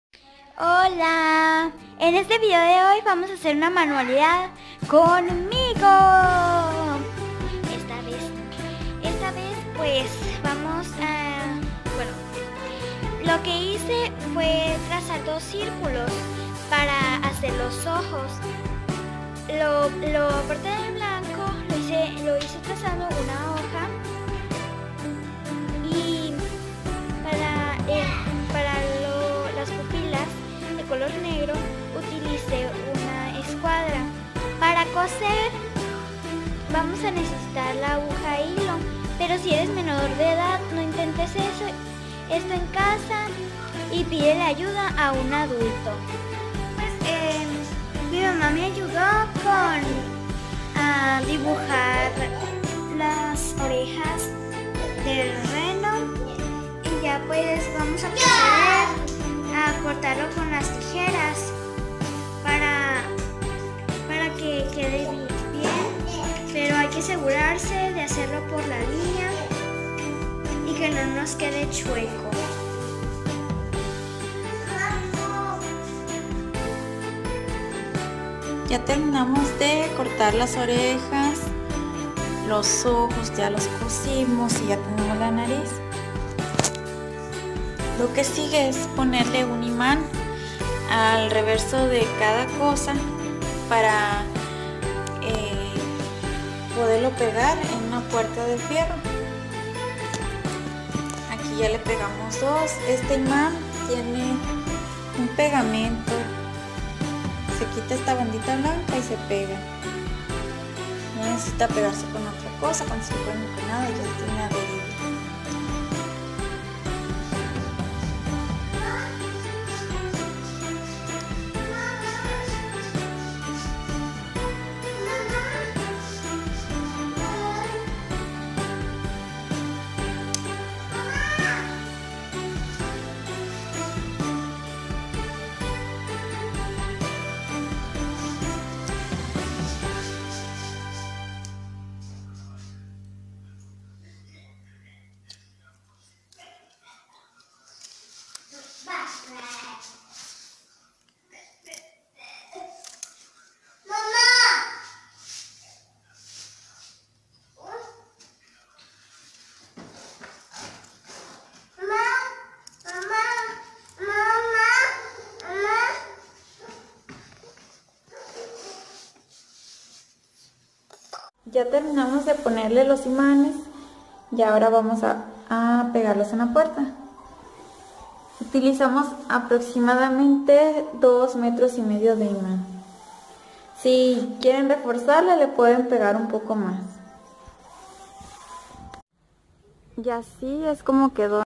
Reno navideño en fieltro para puerta | Paso a paso
Adorno navideño fácil, económico y bonito. Esta hecho con fieltro, también se puede hacer con foamy normal y/o diamantado. Los materiales son:

1 círculo color rojo de 28 centímetros de diámetro.
2 círculos color blanco de 18 centímetros de diámetro.
2 círculos color negro de 9 centímetros de diámetro.
2 cuernos (de la forma que quieras) color café de 60 centímetros de largo.

Los cuernos los dibujé a mano alzada. Para que salgan iguales primero dibuja un cuerno y recortalo, después traza su silueta sobre otro pedazo de fieltro.

Para pegarlos en puerta de metal utilicé 2.5 metros de imán con adherible.

Está inspirado en una imagen que vimos en internet. Si tienes una duda o sugerencia, escribela en los comentarios.